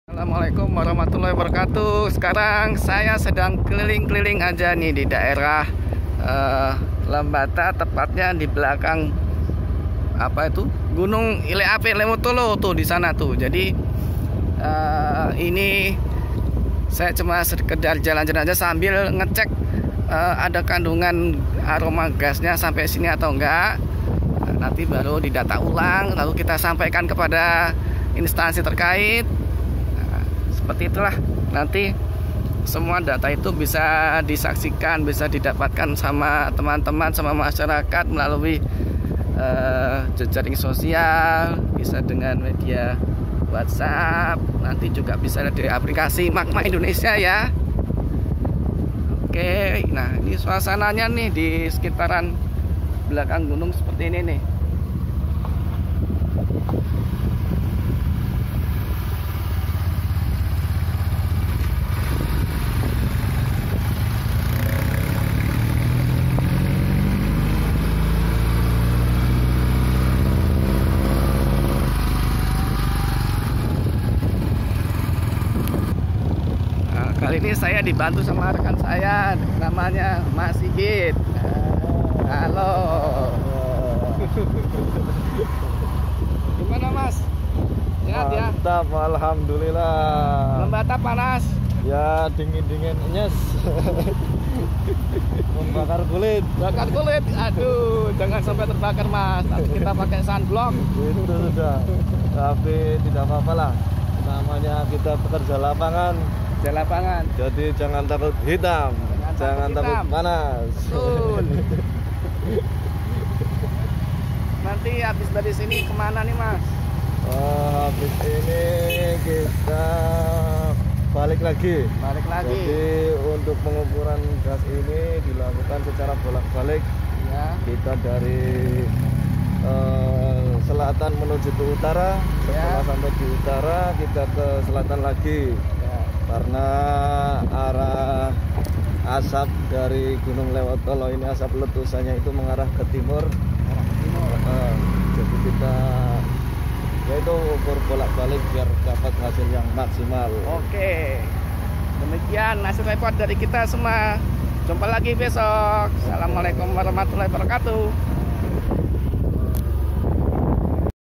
Pesona Nusantara Gunungapi Ile Ape Lewotolok Lembata NTT
Berkeliling ke bagian belakang gunungapi ile ape sambil mengukur kandungan gas